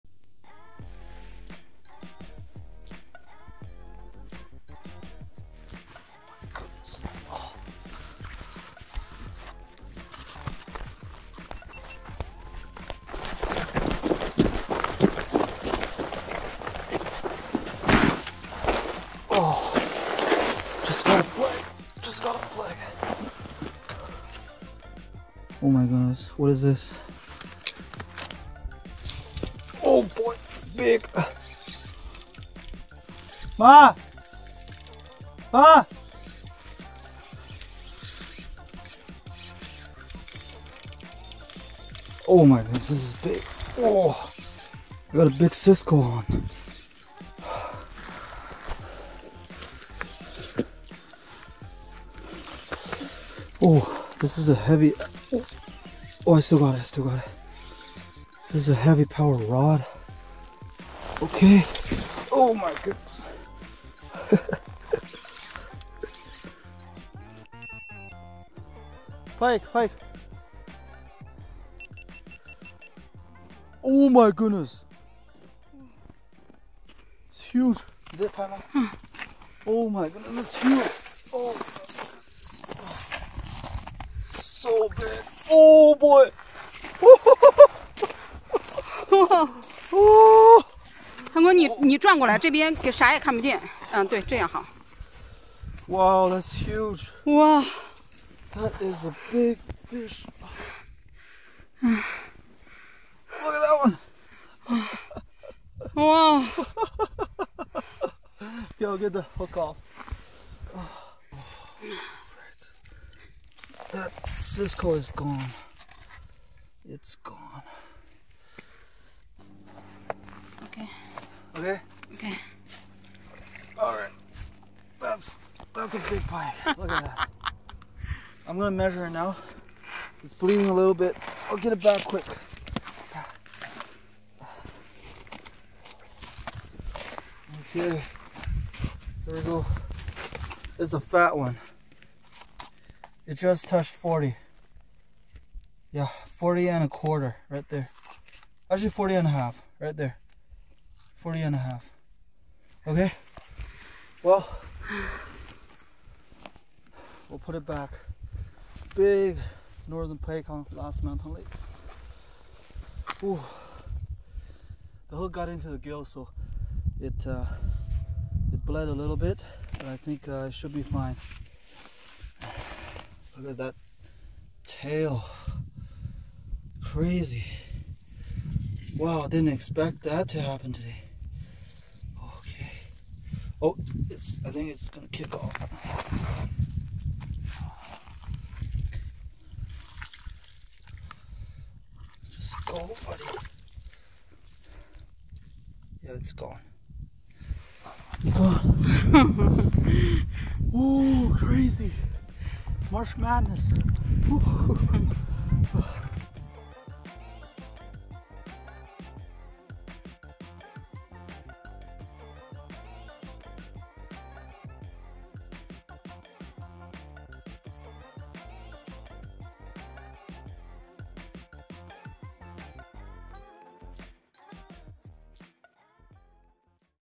New Ice PB Northern Pike on iFish Pro TipUp | March Madness!!
Mom and I were trying to catch burbot at Last Mountain Lake but had no luck. Decided to setup a tipup rig with a 11 inch cisco on the hook during mid day. What an unexpected catch!! New PB Broken!!

AMF Wire Leader Material 20lb

BEARKING Zeus Spinning Reel